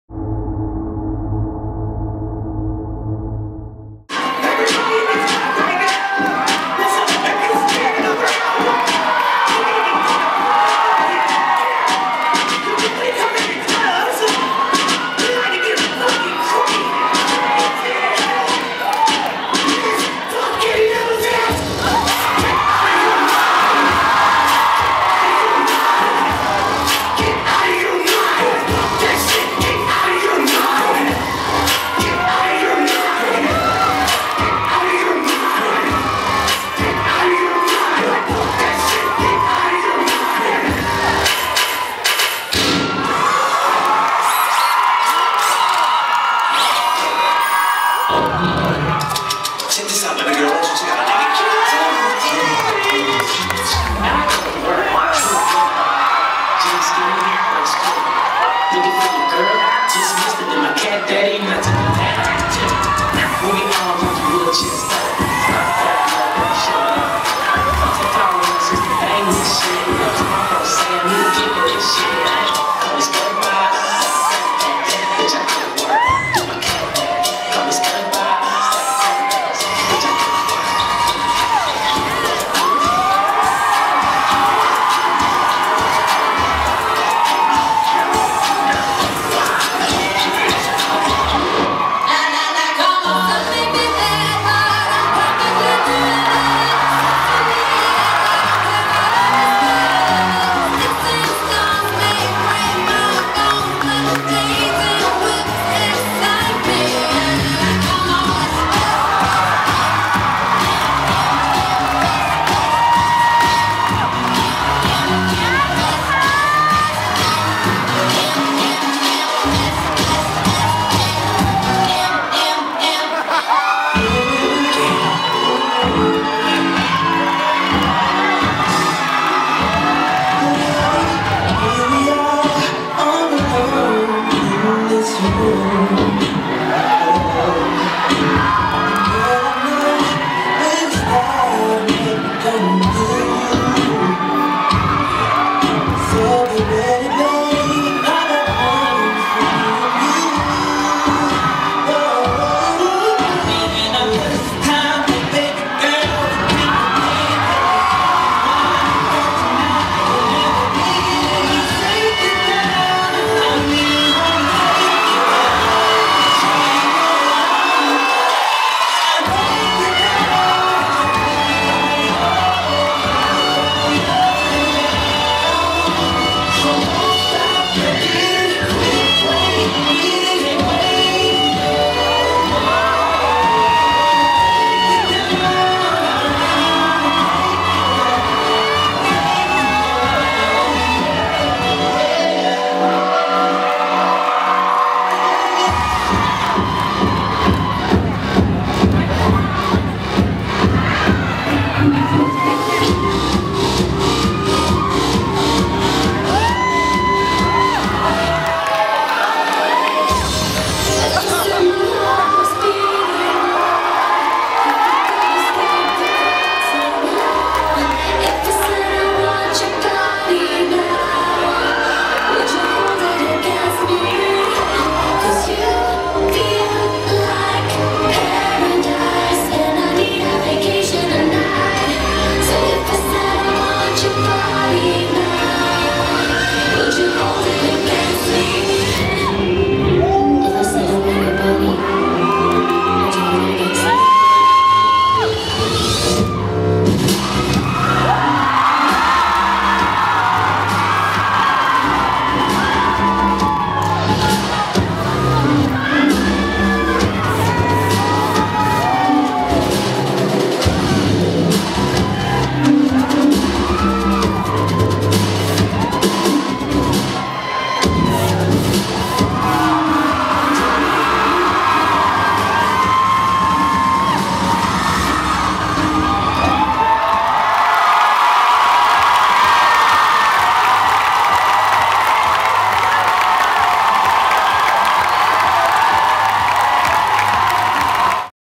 ARROWBANDS 2011: DELTA CHI - First Place (HD)
See Delta Chi take first place (fraternity) in Pi Beta Phi's annual Arrowbands competition at UC Berkeley.